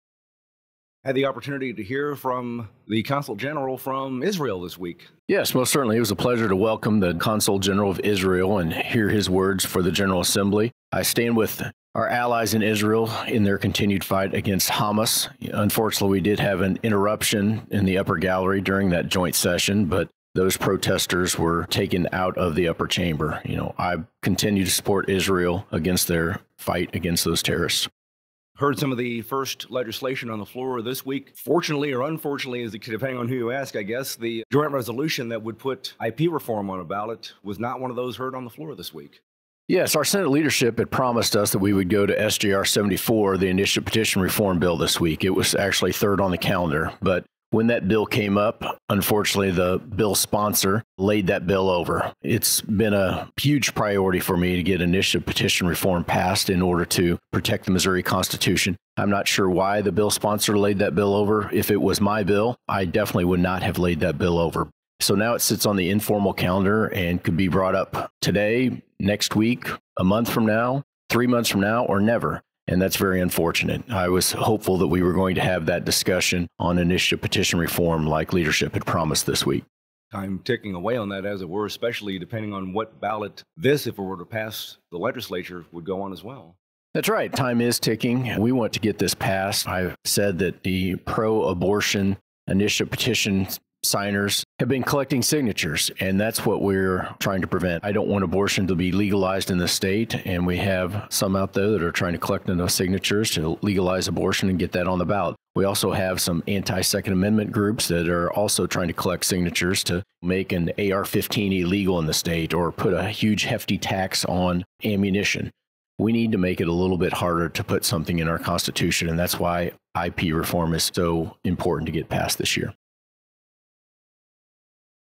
Audio: Missouri Sen. Denny Hoskins' Podcast for the Week of Feb. 5, 2024
JEFFERSON CITY — State Sen. Denny Hoskins, R-Warrensburg, discusses his thoughts on the address from the Israeli Consul this week and Senate Joint Resolutions 74, 48, 59, 61 & 83, which would let voters decide on changes to how initiative petitions would go on future Missouri ballots.

00:01 - Big presentation for lawmakers
00:28 - SJRs 74, 48, 59, 61 & 83